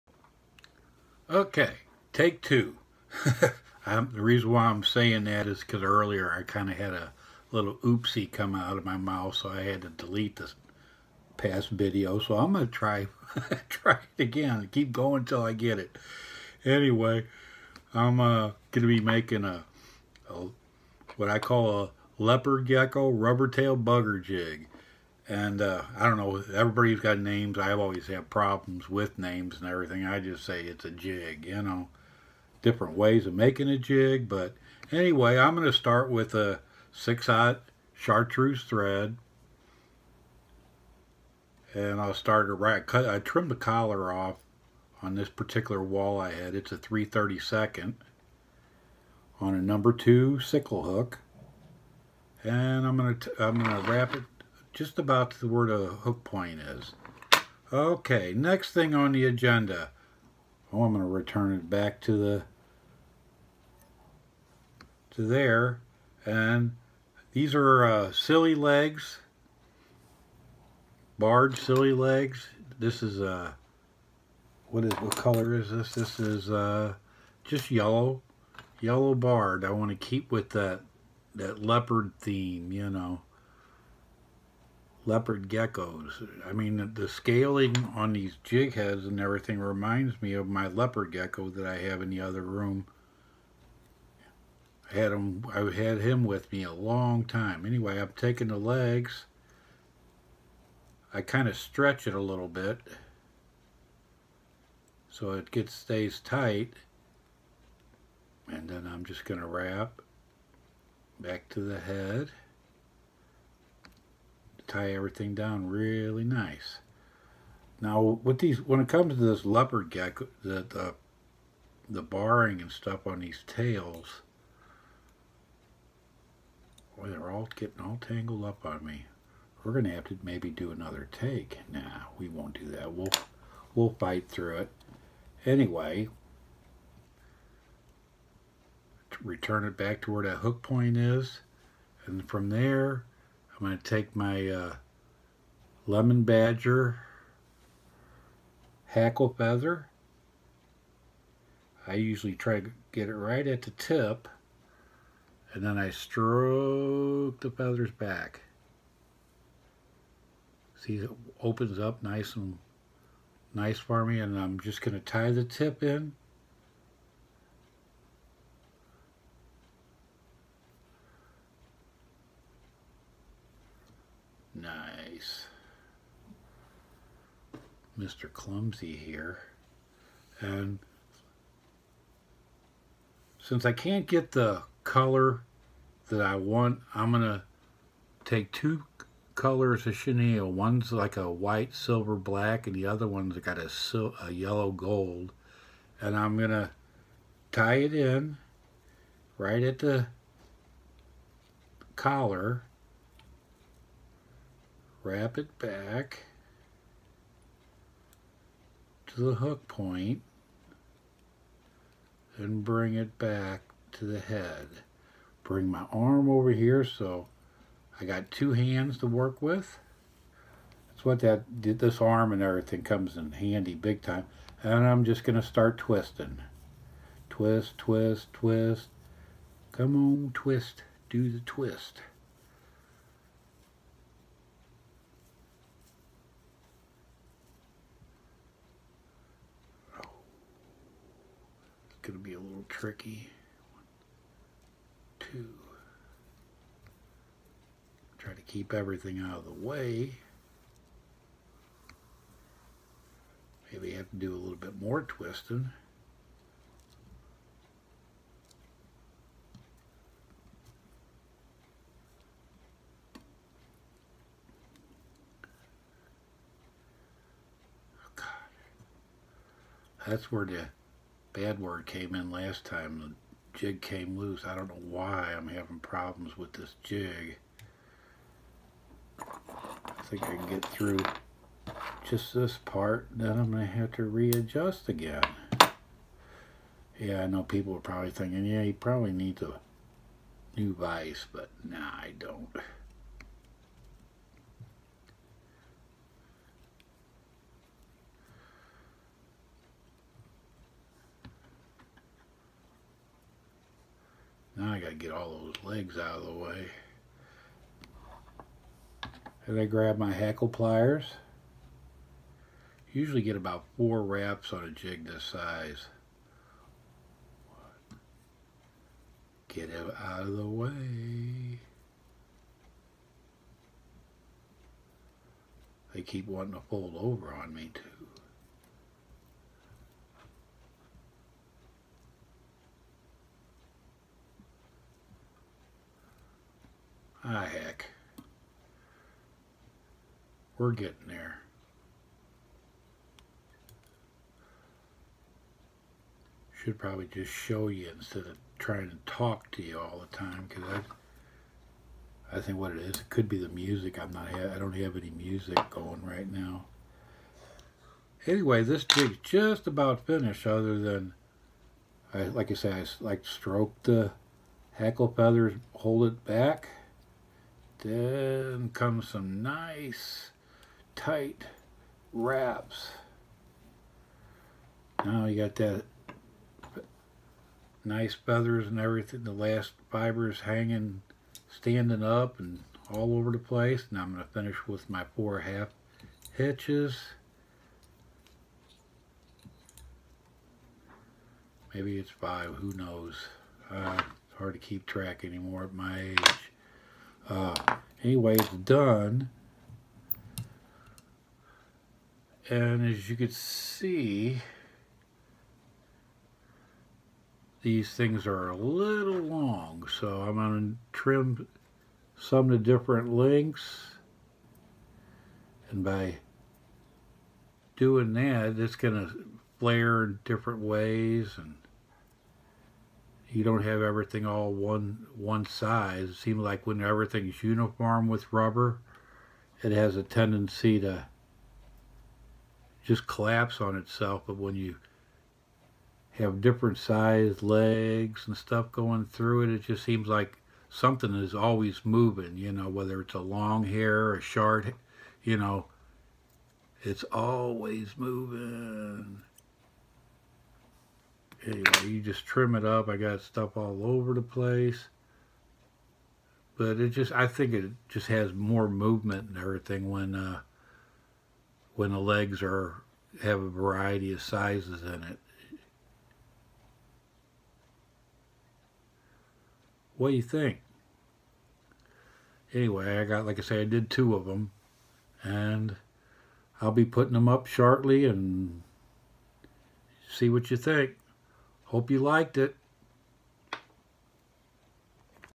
Rubber tail crappie bugger jig
I’m making a rubber tail crappie bugger jig in leopard gecko color using yellow barred sili legs, yellow badger hackle and two colors of chenille that I twisted together to get the body color that I need for this jig. Jig is a 3/32 walleye head on a #2 sickle hook with 4 mm 3-D eyes.